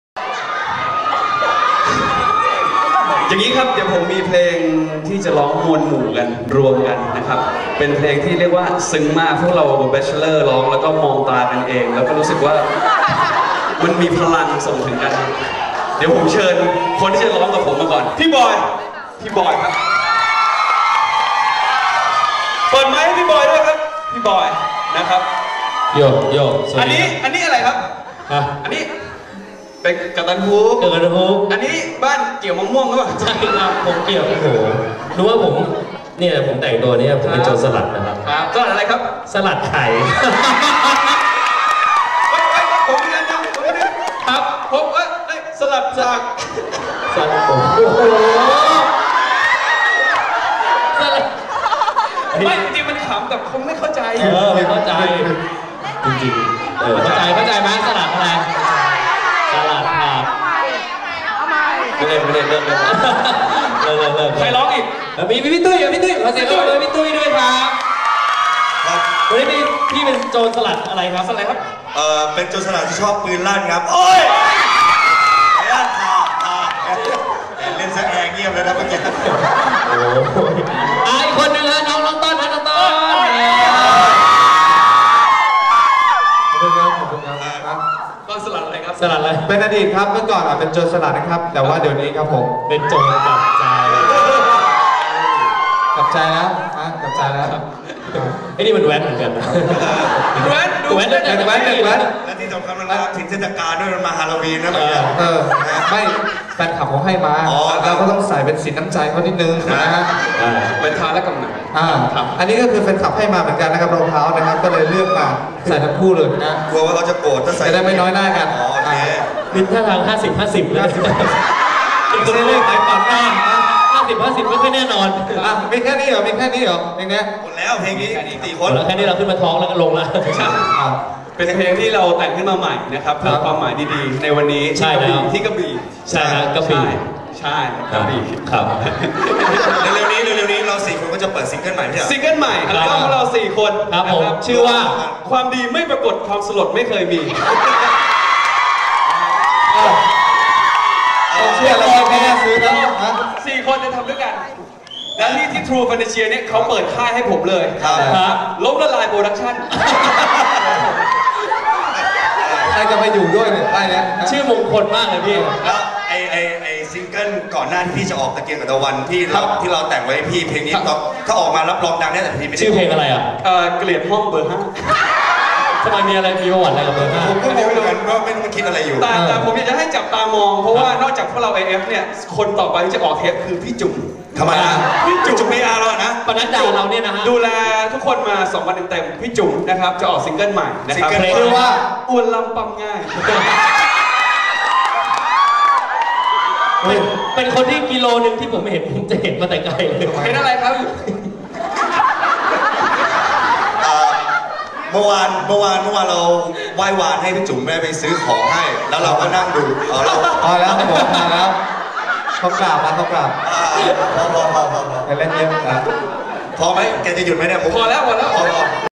ตุ้ย ว่าน บอย ต้อล - Talk Show | Pirate Night Concert in Krabi
ตุ้ย และ หนุ่มๆ Bachelors ใน Pirate Night Concert : Cool Bachelor in Krabi 7/11/2552